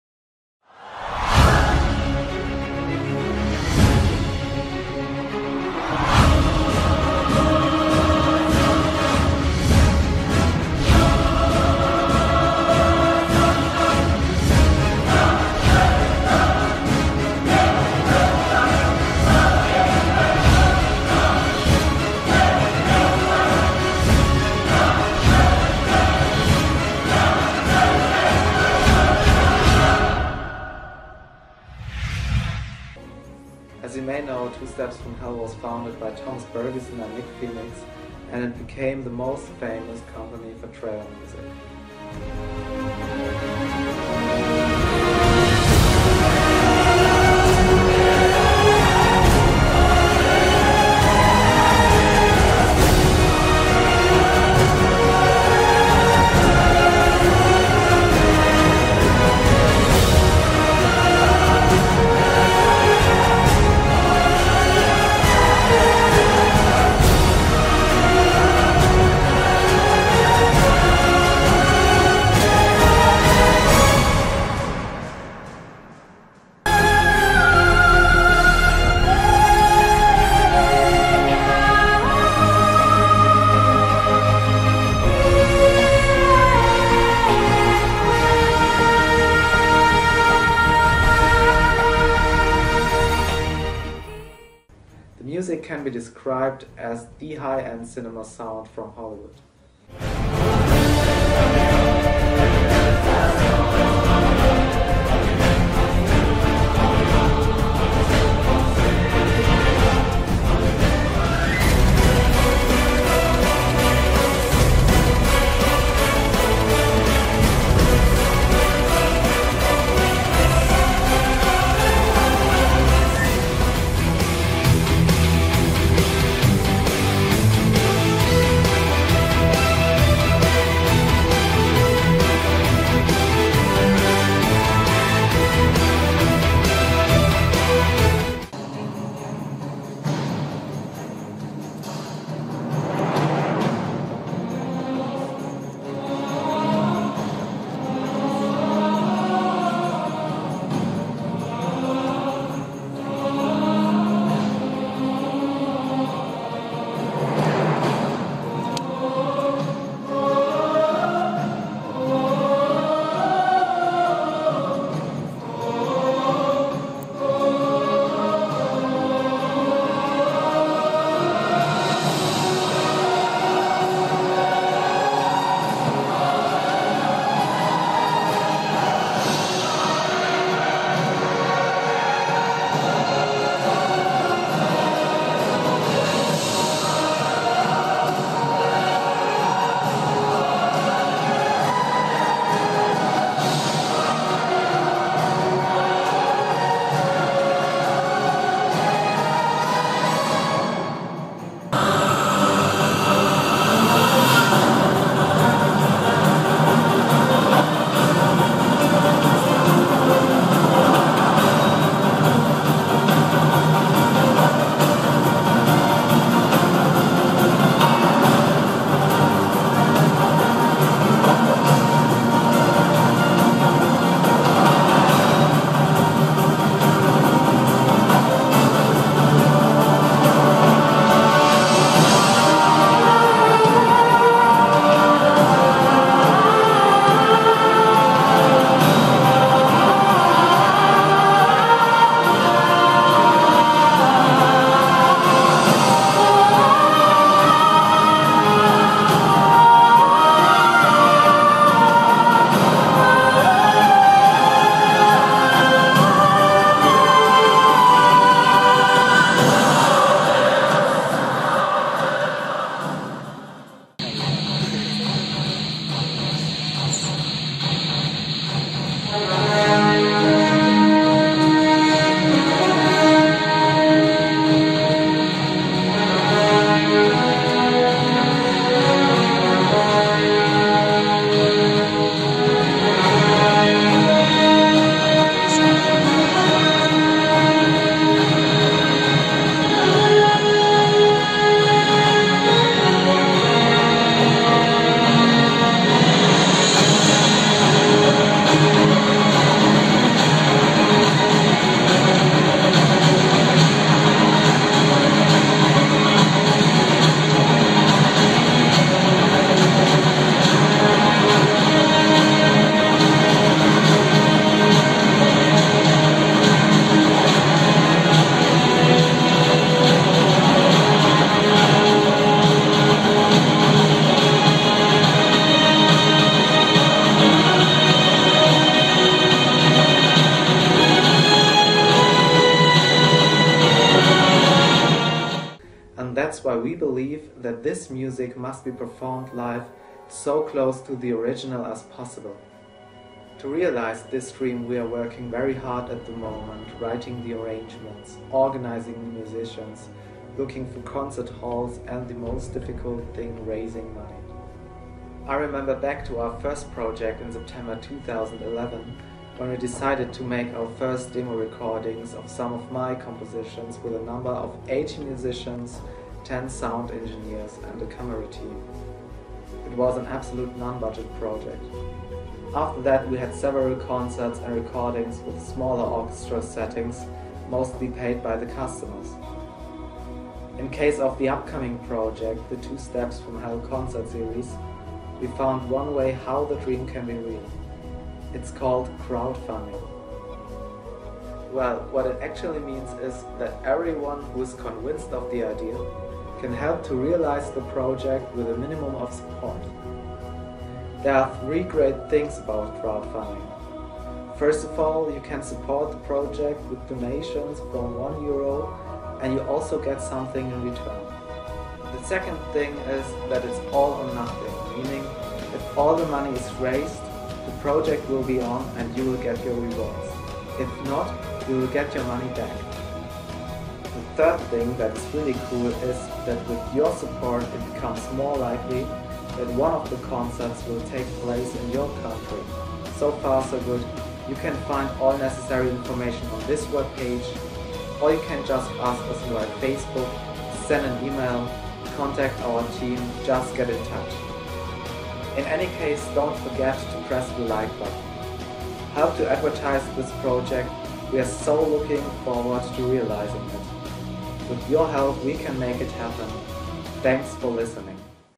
Two Steps From Hell: Trailer concert at the Walt Disney 2013-2014
During the concert of the band "Two Steps From Hell" is not record why the video quality is low.
Credits: Xiphon Audio